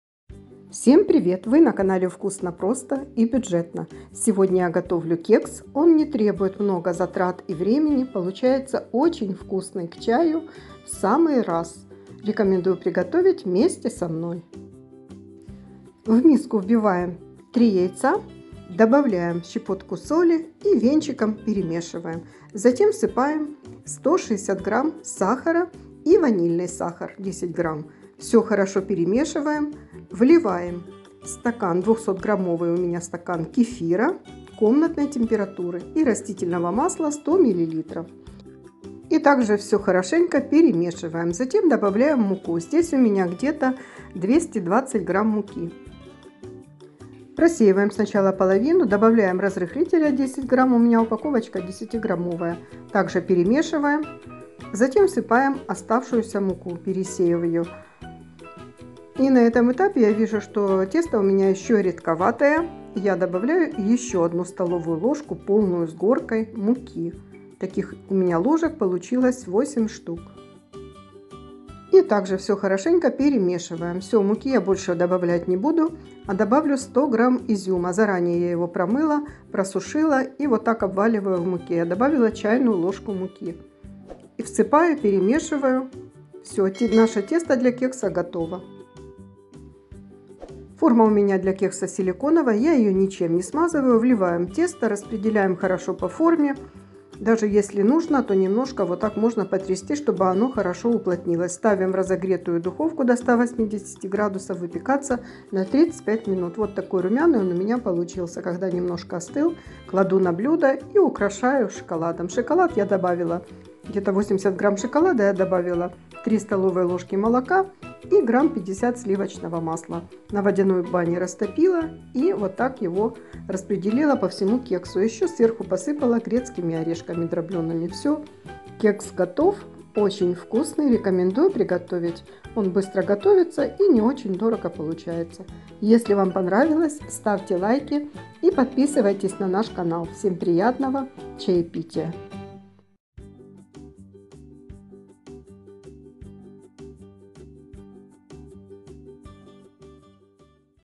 Кекс с изюмом на кефире! Очень простой, вкусный и бюджетный рецепт!
Сегодня готовлю бюджетный кекс, без сметаны и сливочного масла, получается очень вкусно!

Для приготовления мне понадобилось:

Масло растит.- 100 мл.
Изюм - 100 г. + 1 ч.л. муки для
обваливания

В глубокую посуду вбиваем яйца, добавляем щепотку соли, сахар, ванильный сахар, перемешиваем венчиком, максимально растворив сахар, вливаем кефир,(все продукты должны быть комнатной температуры), растительное масло, перемешиваем до однородности и всыпаем муку в два приёма и разрыхлитель. Все смешиваем венчиком.
Изюм заранее помыла, залила кипятком на 15 минут, затем высушила бумажным полотенцем.
Кладем в мисочку и посыпаем 1 ч.л. муки, хорошо обваливаем и кладём в тесто, перемешиваем лопаточкой и тесто готово.
Переливаем тесто в форму для кексов, если нет большой формы, можно выпекать в маленьких, время для выпечки понадобится меньше.
У меня большая форма, поместилось всё тесто, хорошо распределяем тесто по форме, можно немного потрясти, чтобы не было пустот.
Ставим в разогретую духовку до 180°, выпекаться на 35 минут, готовность проверяем шпажкой. Если формочки маленькие , то достаточно 20 минут, а там смотрите по своей духовке.

Дорогие друзья!!! Очень надеюсь, что Вам понравилось видео!

Если у кого есть возможность
Можете Поддержать проект через карту банка:
ПриватБанк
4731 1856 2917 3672  Василенко Наталия